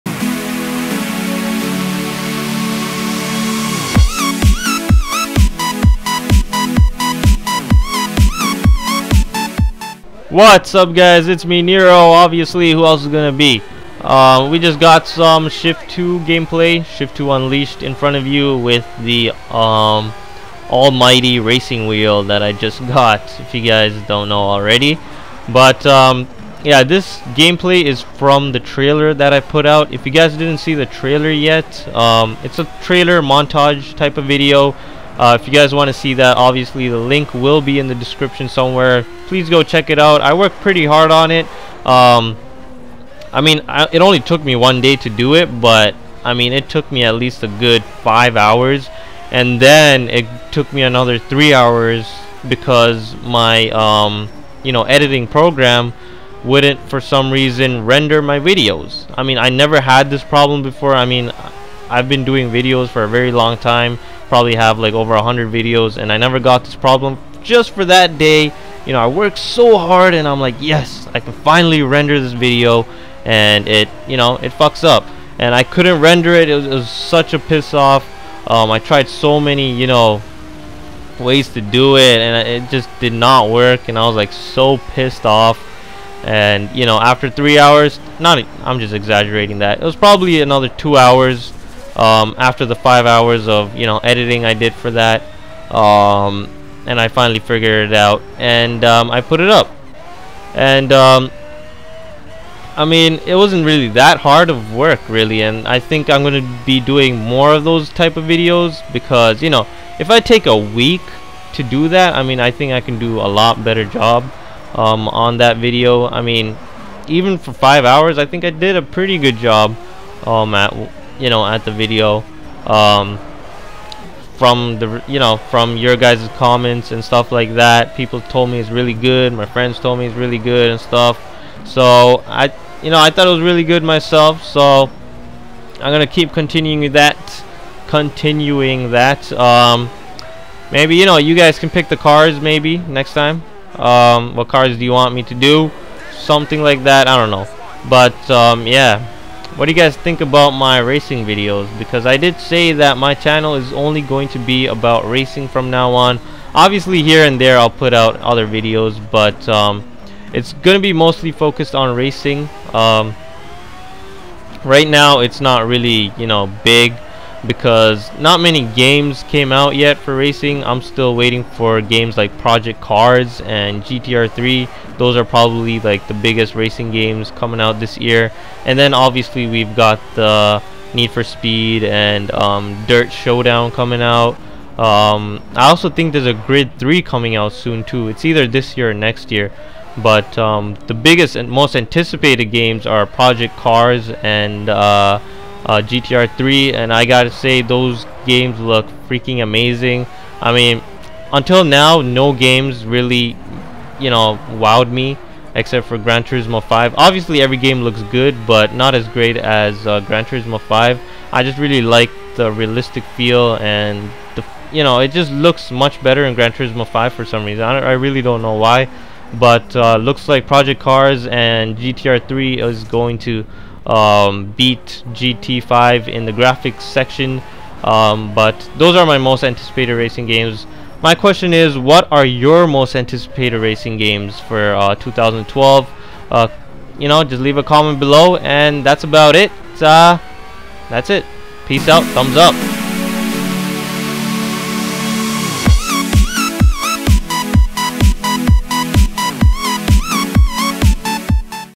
Whats Your Most Anticipated Racing Game? GTR3? Project Cars?
Thumbs up for more videos to come!

In this video I will be talking about a bit of what my most anticipated racing game to come out. Check out video for more!

Hope you guys enjoyed this!